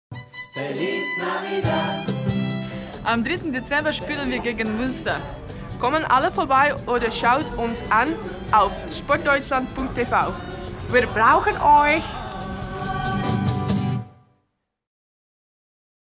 Rote Raben Adventskalender - Türchen 2
Der musikalische Adventskalender der Roten Raben und ISAR TV. Heute mit Marlies, Ilka und Anja und Türchen Nummer 2.